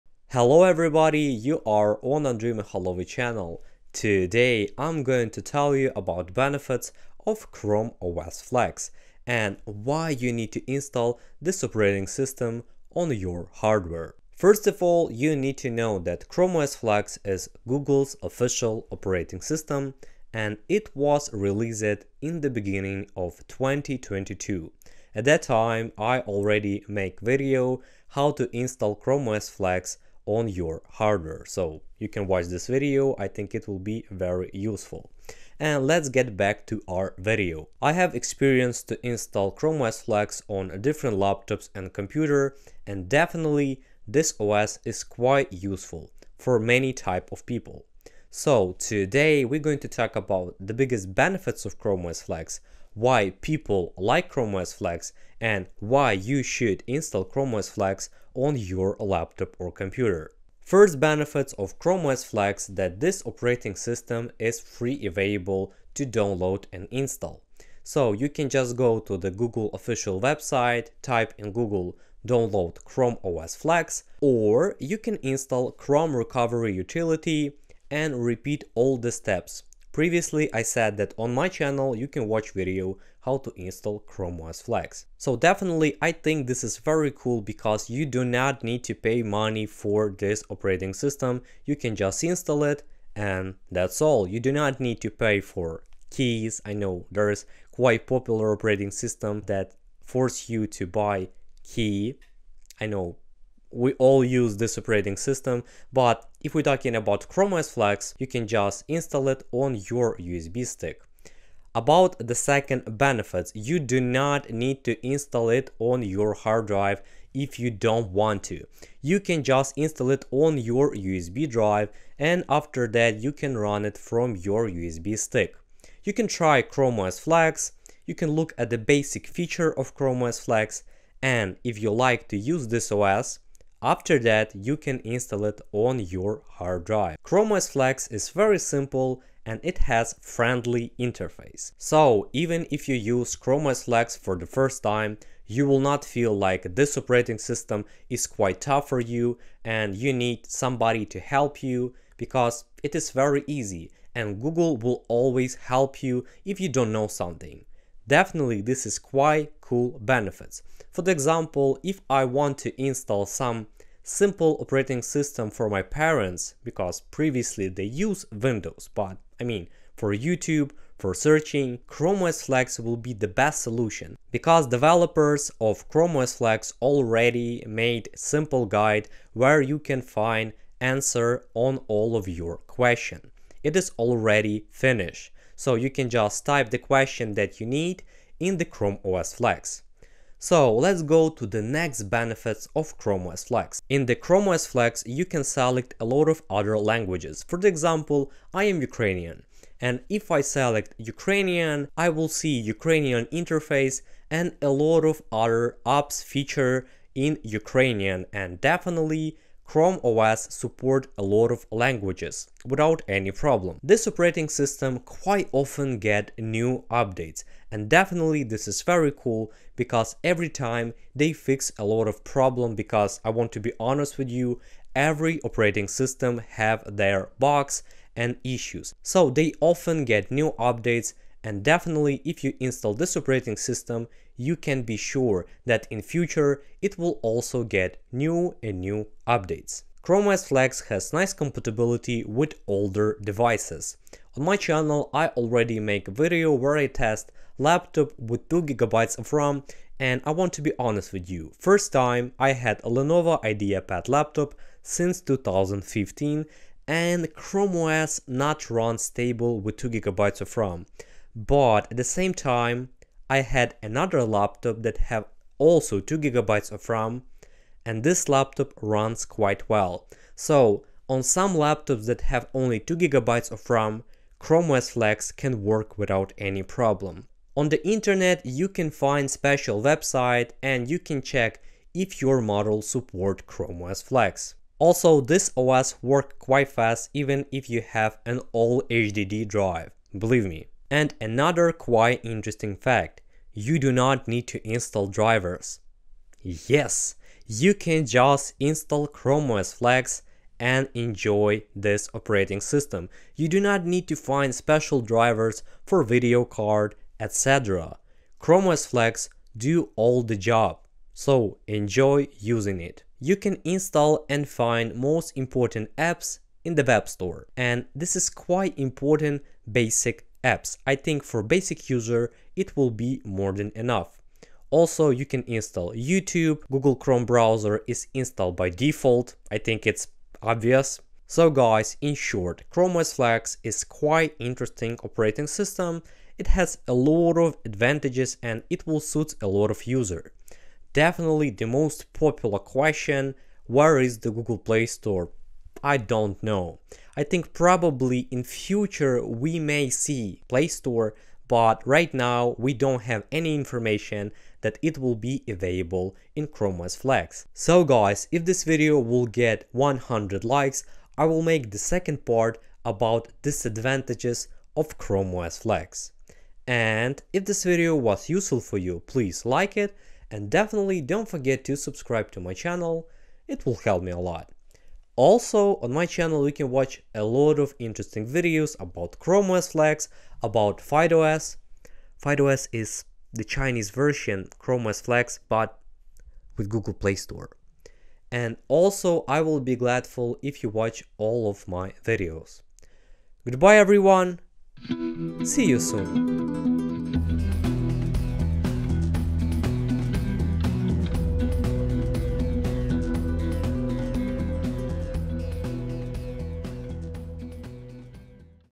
Why YOU Should TRY Chrome OS Flex
00:00 Introduction
01:00 Chrome Os Flex is Free
01:50 You can run it from a USB drive
02:17 Flex OS is Friendly
03:33 New updates
03:59 Support old devices
05:12 Install Apps from Web-Store
05:40 Where is Play Store?

Chrome OS Flex is even Good without a Play Store!
Today I'm going to tell you about the benefits of Chrome OS Flex.
This OS is very popular and I decided to share my thoughts about it.

👋Links
Check if your device is officially compatible: